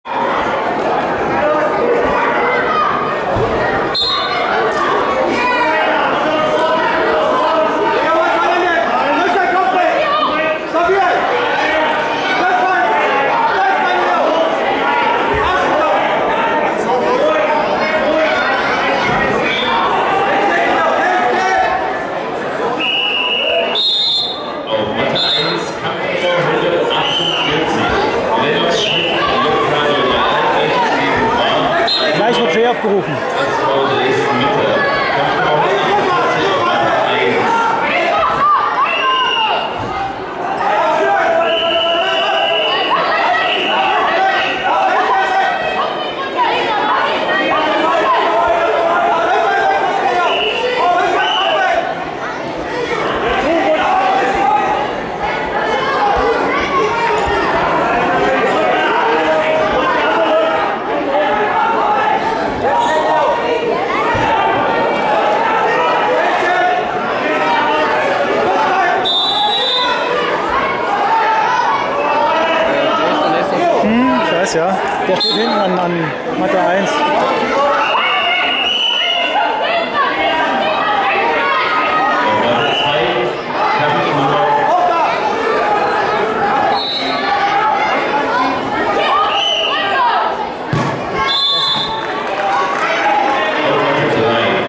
Merlin Fritzsche (RV Eichenkranz Lugau) - Neo-Fabrice Heilmann (SV Luftfahrt Ringen) D/42kg 2019
Offene Mitteldeutsche Meisterschaften Pößneck, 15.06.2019